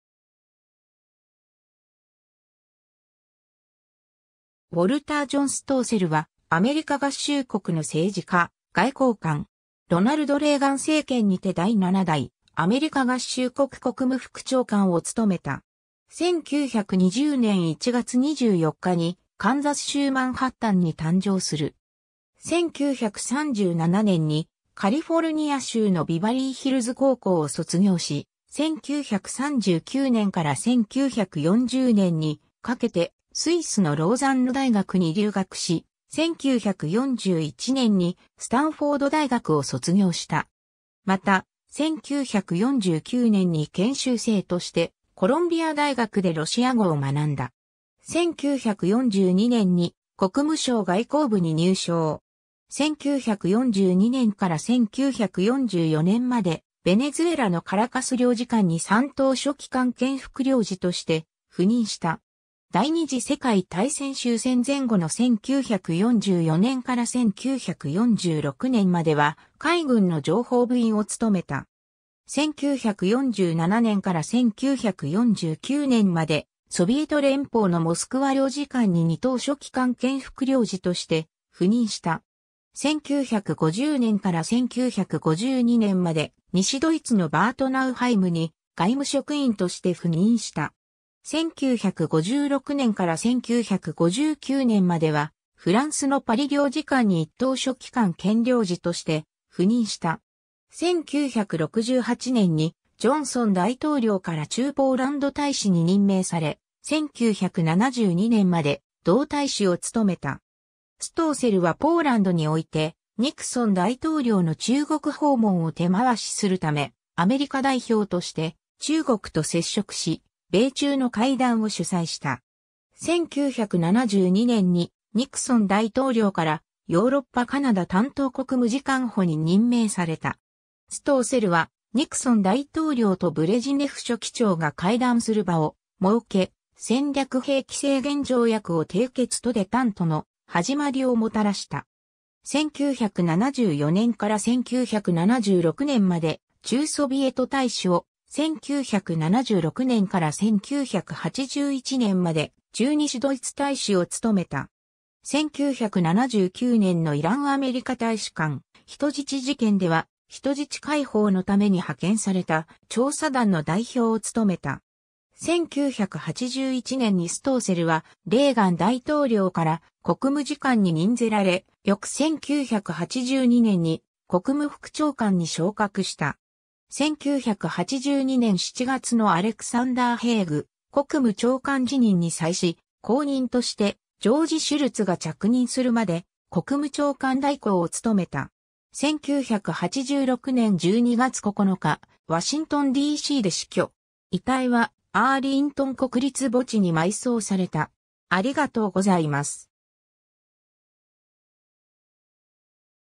ウォルター・ジョン・ストーセル
アメリカ合衆国国務次官（政治担当）
ウォルター・ジョン・ストーセル（英語：Walter John Stoessel、1920年1月24日 - 1986年12月9日）は、アメリカ合衆国の政治家、外交官。
ロナルド・レーガン政権にて第7代アメリカ合衆国国務副長官を務めた。
 1920年1月24日にカンザス州マンハッタンに誕生する。
1937年にカリフォルニア州のビバリーヒルズ高校を卒業し、1939年から1940年にかけてスイスのローザンヌ大学に留学し、1941年にスタンフォード大学を卒業した。
また、1949年に研修生としてコロンビア大学でロシア語を学んだ。
 1942年に国務省外交部に入省。
1942年から1944年までベネズエラのカラカス領事館に三等書記官兼副領事として赴任した。
第二次世界大戦終戦前後の1944年から1946年までは海軍の情報部員を務めた。
 1947年から1949年までソビエト連邦のモスクワ領事館に二等書記官兼副領事として赴任した。
1950年から1952年まで西ドイツのバート・ナウハイムに外務職員として赴任した。
1956年から1959年まではフランスのパリ領事館に一等書記官兼領事として赴任した。
 1968年にジョンソン大統領から駐ポーランド大使に任命され、1972年まで同大使を務めた。
ストーセルはポーランドにおいてニクソン大統領の中国訪問を手回しするため、アメリカ代表として中国と接触し、米中の会談を主催した。
 1972年にニクソン大統領からヨーロッパ・カナダ担当国務次官補に任命された。
ストーセルはニクソン大統領とブレジネフ書記長が会談する場を設け、戦略兵器制限条約を締結とデタント（緊張緩和）の始まりをもたらした。
1974年から1976年まで駐ソビエト大使を、1976年から1981年まで駐西ドイツ大使を務めた。
1979年のイランアメリカ大使館人質事件では、人質解放のために派遣された調査団の代表を務めた。
 1981年にストーセルはレーガン大統領から国務次官（政治担当）に任ぜられ、翌1982年に国務副長官に昇格した。
1982年7月のアレクサンダー・ヘイグ国務長官辞任に際し、後任としてジョージ・シュルツが着任するまで、国務長官代行を務めた。
 1986年12月9日、ワシントンD.C.で死去。
遺体はアーリントン国立墓地に埋葬された。